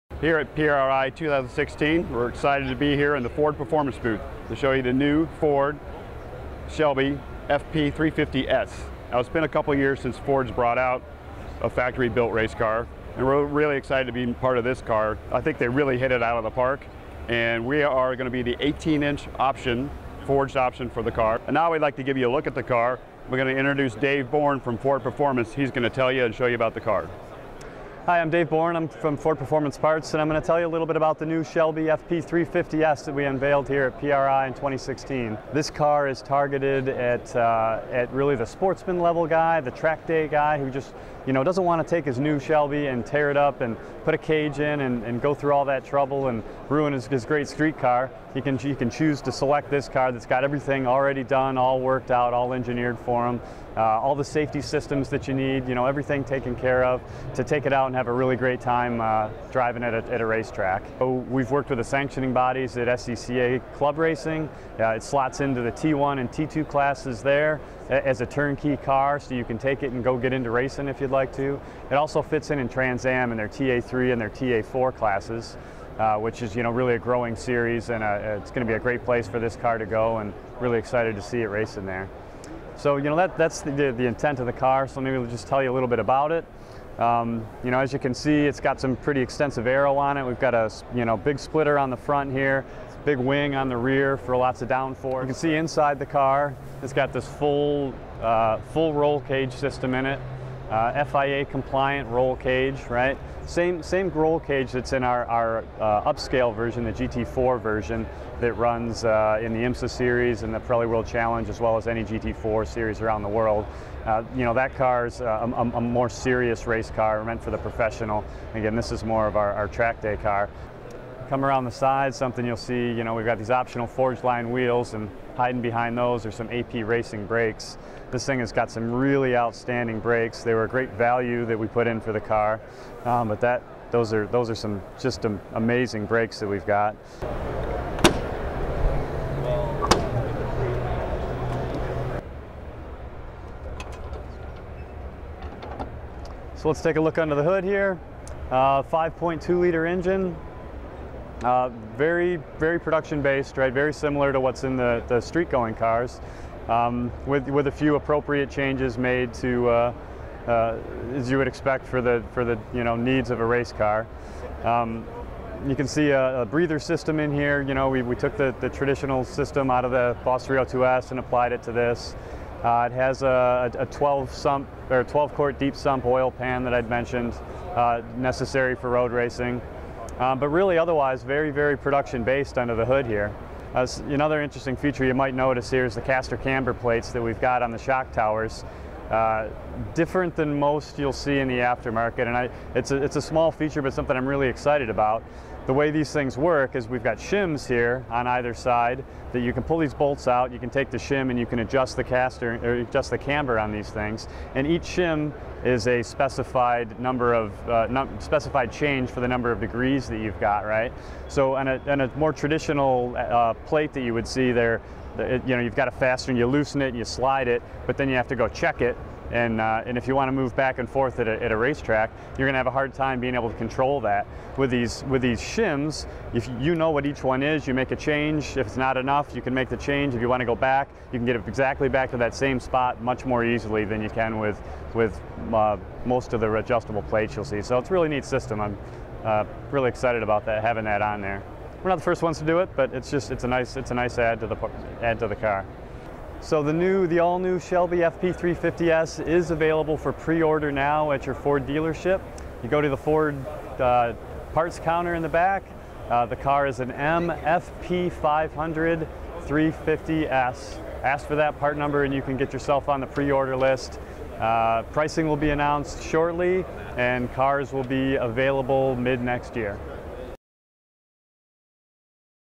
Ford's Shelby FP350R Racecar Debuts at the 2016 PRI Show on Forgeline GS1R Wheels
Want to know more about Ford’s latest factory-direct racecar? At last weekend’s 2016 PRI Show, Ford Performance Engineering Manager, Dave Born, gave us a closer look, at the new Ford Shelby FP350R Mustang! Ford’s new FP350S is engineered for competition in Trans Am, NASA, and SCCA club racing events, is powered by a 5.2-liter 32-valve V8 engine (prepared by Ford Performance Parts) and arrives equipped with a seam-welded modified unibody, six-point FIA-compliant roll cage, Sparco FIA-compliant racing seat, quick-release steering wheel, Ford Performance splitter, adjustable carbon fiber rear wing, MOTEC data acquisition system, race-tuned suspension & steering, and optional 18x11 Forgeline one piece forged monoblock GS1R wheel package!